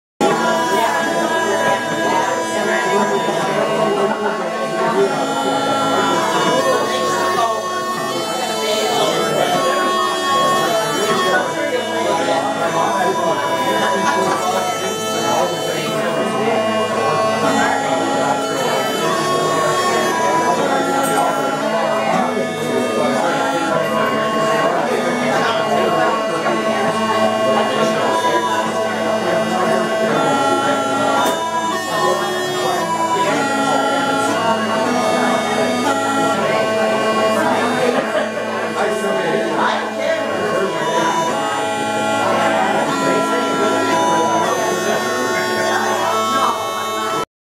da pipes is callin'
alas, camera card was full..a short after hours session as part of the Celtic Colours Festival 2018 at the Highlands Museum , Cape North , Nova Scotia